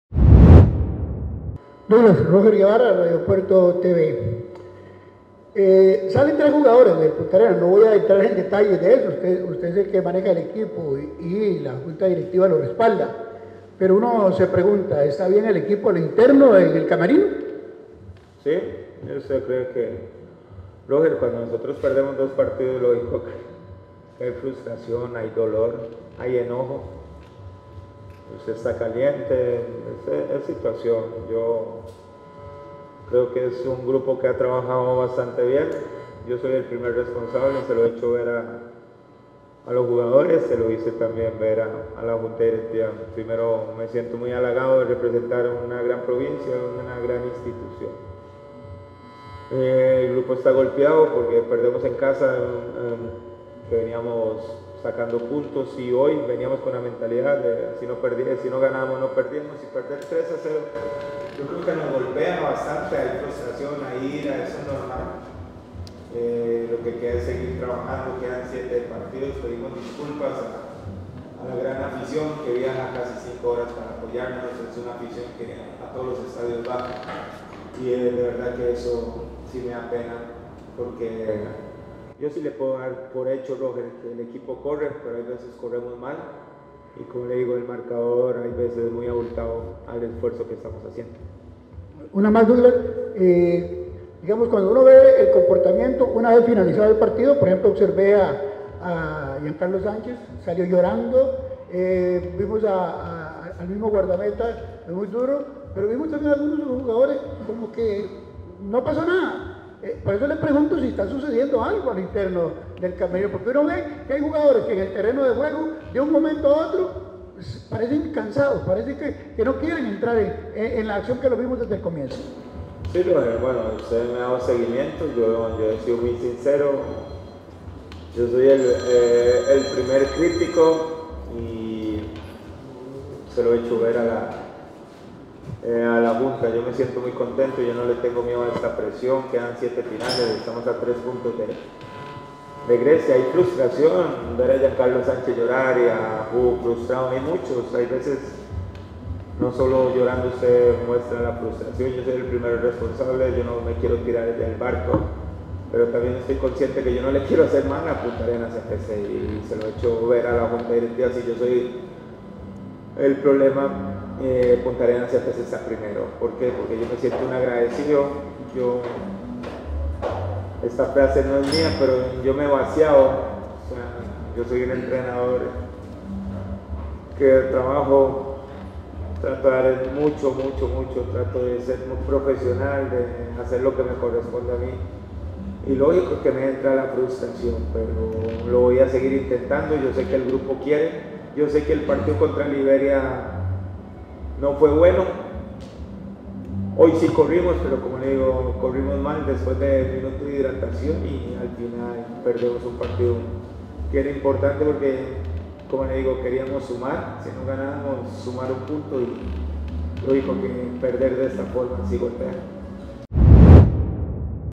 Declaraciones de Douglas Sequeira, Director Técnico de Puntarenas FC
LaFrase l “Yo no le tengo miedo a la presión de estar a tres puntos de Grecia”, Douglas Sequeira, Director Técnico de Puntarenas FC